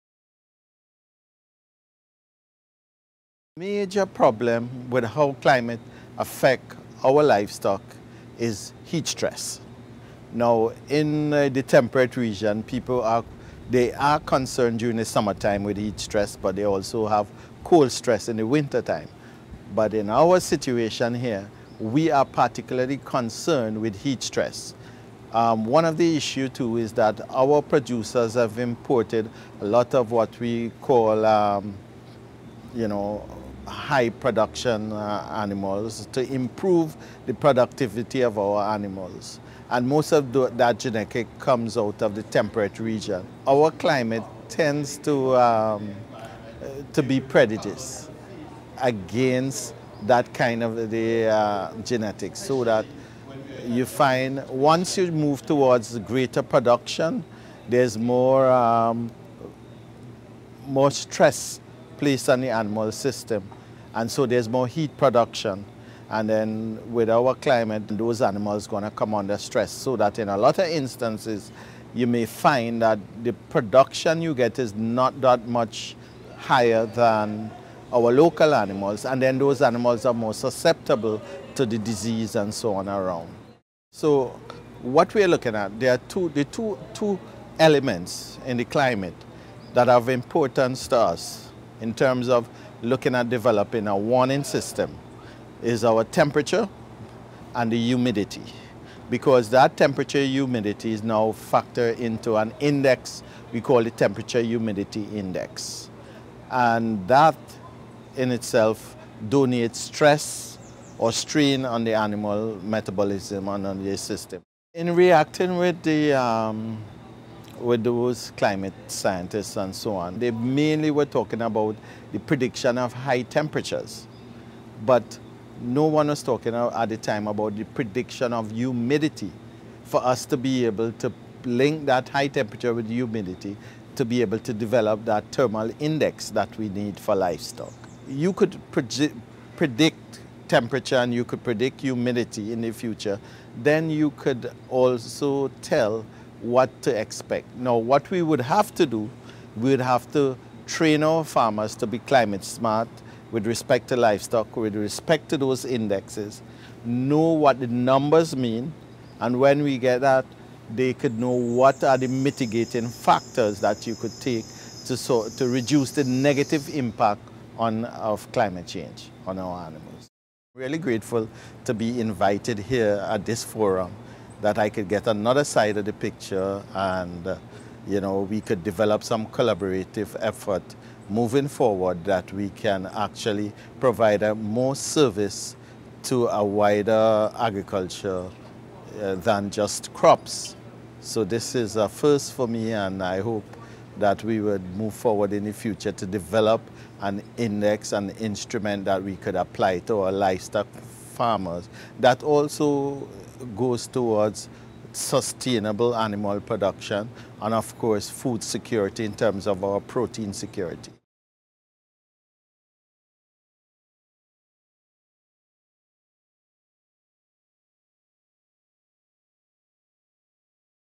New Research: A Heat Index for Livestock
From the 2015 Dry Season Caribbean Climate Outlook Forum, held in November 2015 in St. Kitts.